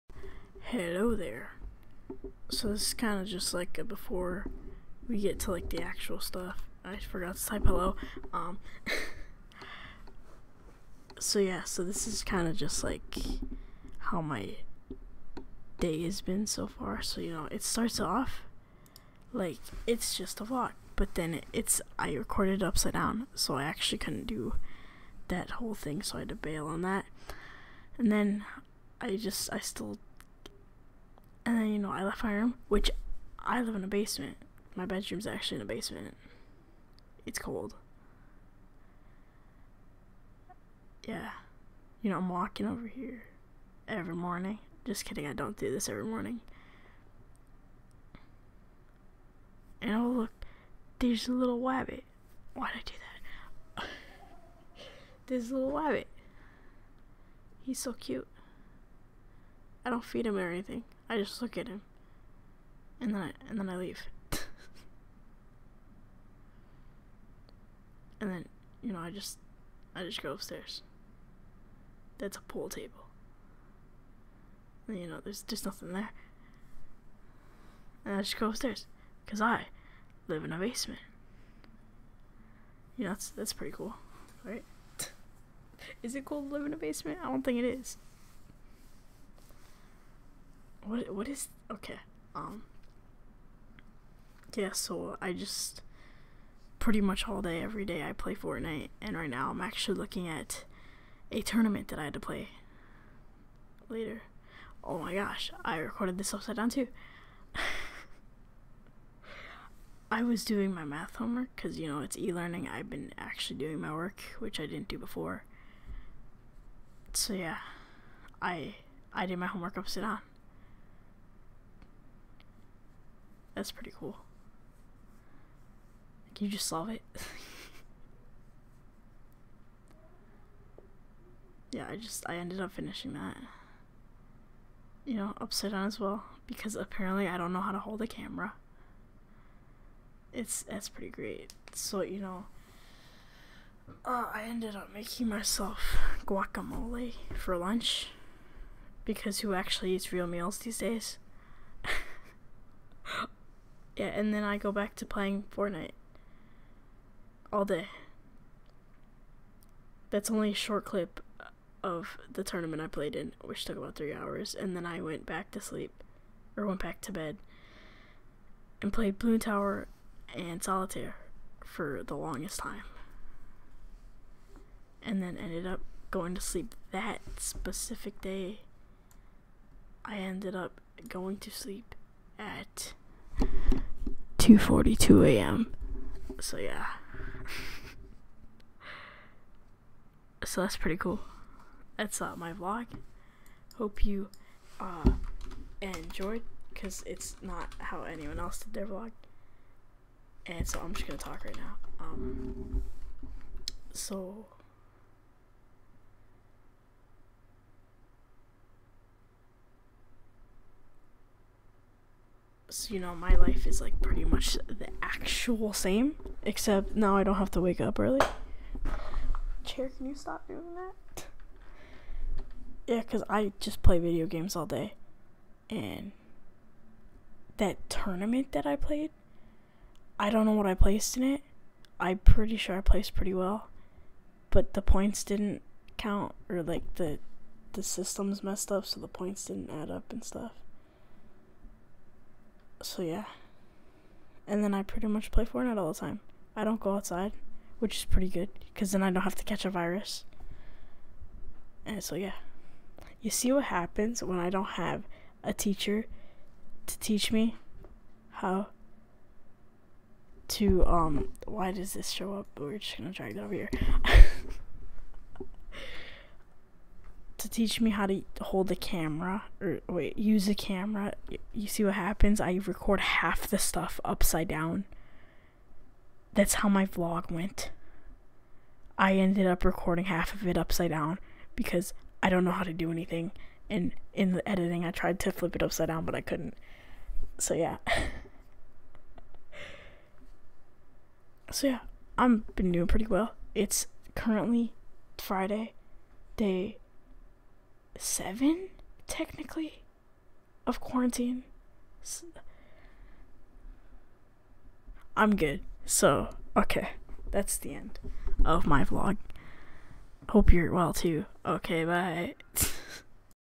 Paulina's Vlog (Life in Quarantine)
Paulina shares what life is like in quarantine and with e-learning. (VP 1 - Spring 2020)